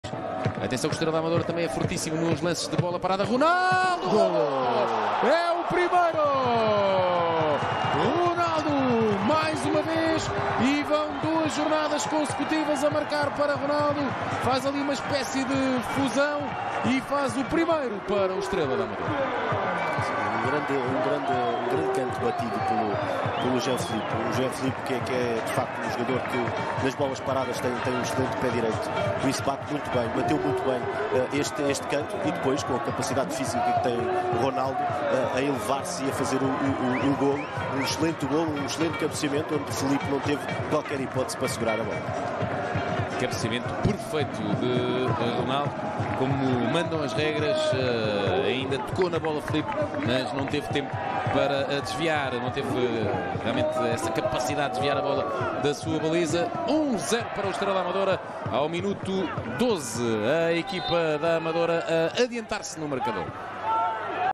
Golo Ronaldo Tavares: Estrela Amadora (1)-0 FC Penafiel - Liga Portugal SABSEG | SPORT TV
Estrela Amadora: Golo de Ronaldo Tavares!